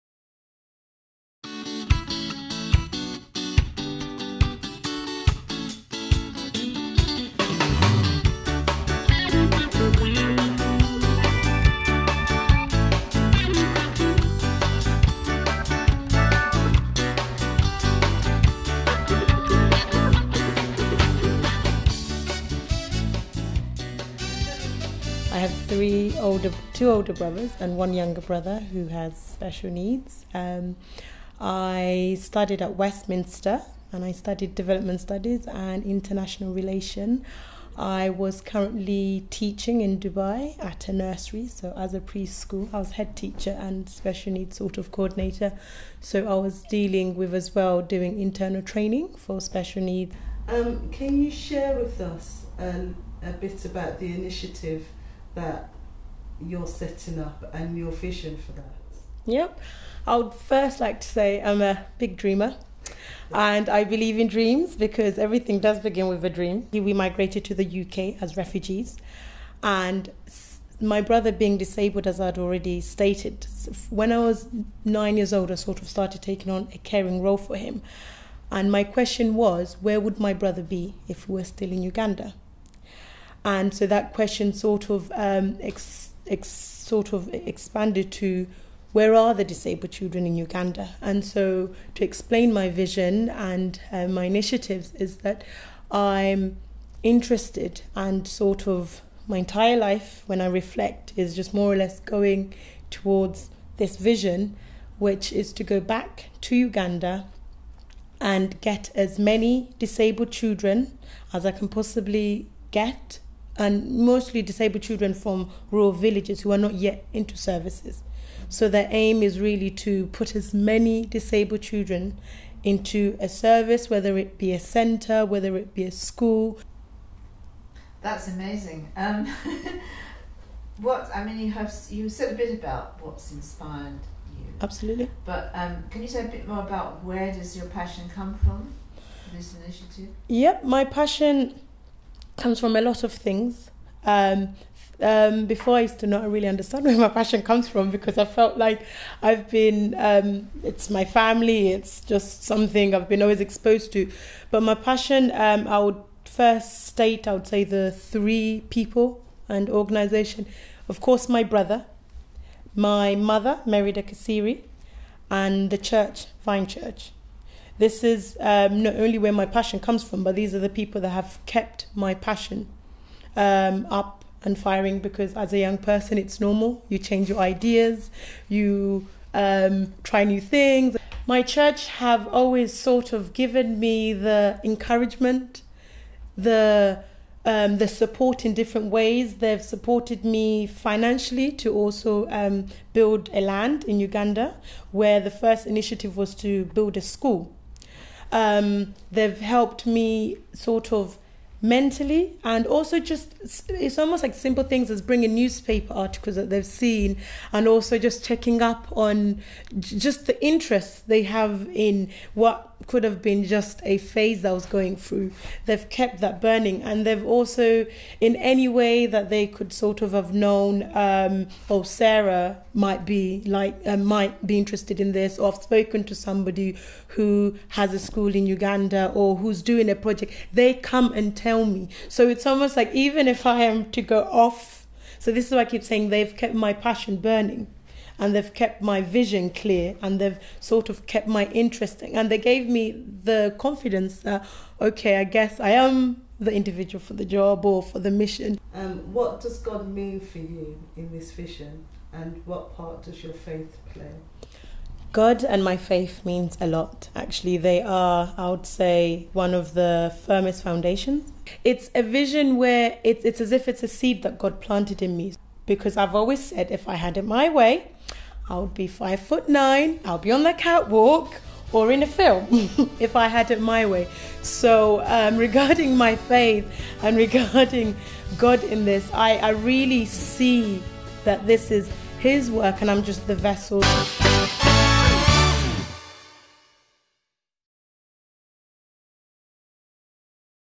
Dream Stories: Sarah Kigozi from Uganda
At Carr's Lane in Birmingham, The United Reformed Church enjoyed a magnificent multicultural event called 'Drinking from our own Wells'. This was a celebration of the joy  that springs from recognising, encouraging and integrating the rich diversity of Christian spirituality, in honour of Jesus.